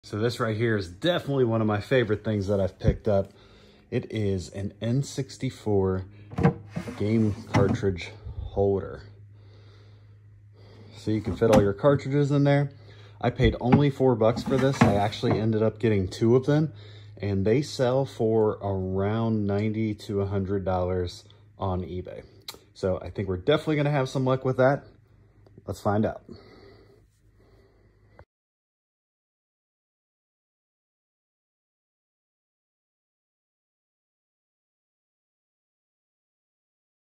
Gaming Thrift Store Finds - N64 Cartridge Holder - Part 5
Game reselling can still be profitable.